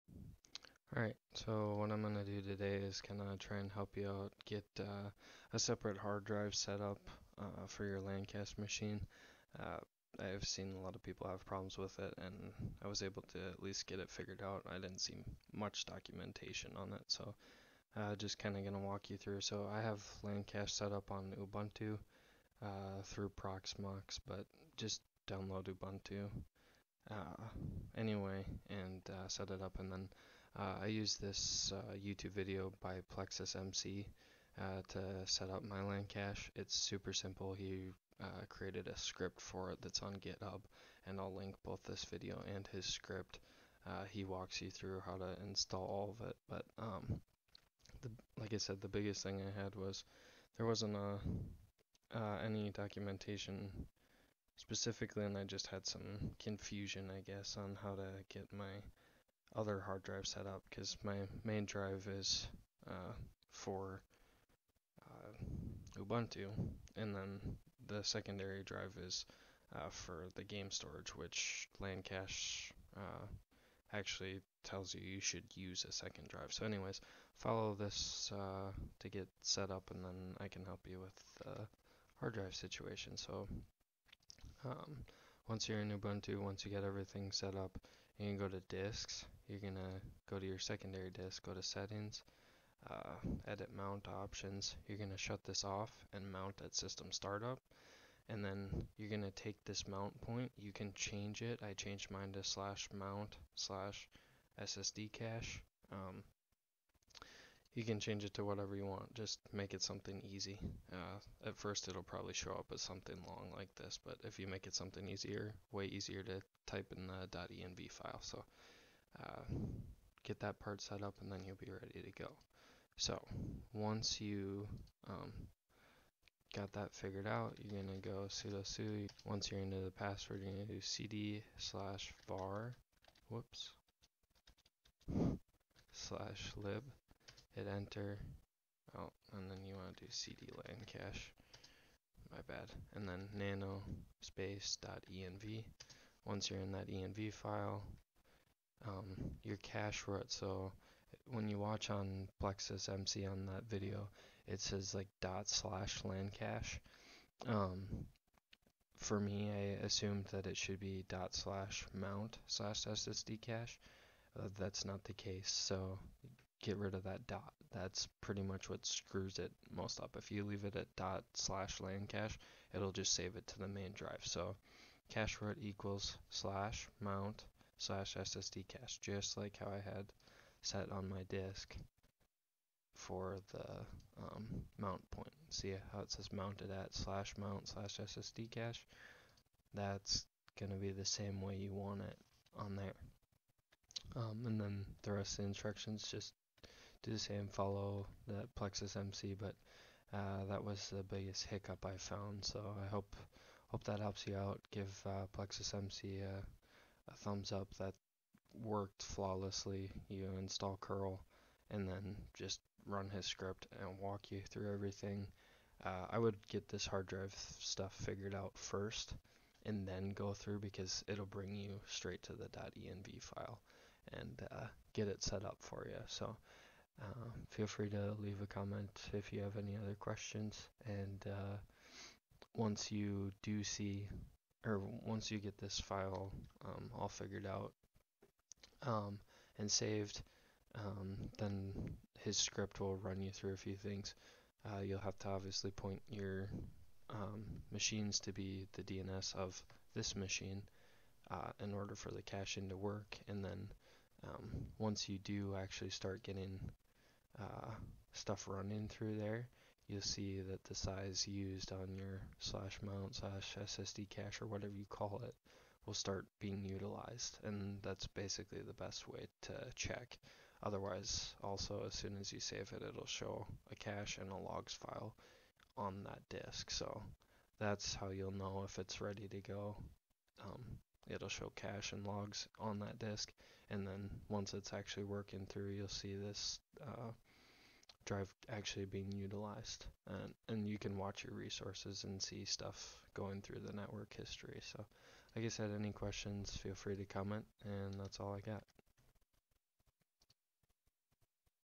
Adding A Secondary Hard Drive to LanCache
I had some problems  getting a secondary drive setup with LanCache. I wasn't able to find much documentation and also found someone having the same problem. I thought I would help and make a video showing how to do it. Let me know if you have any questions! I would love to help.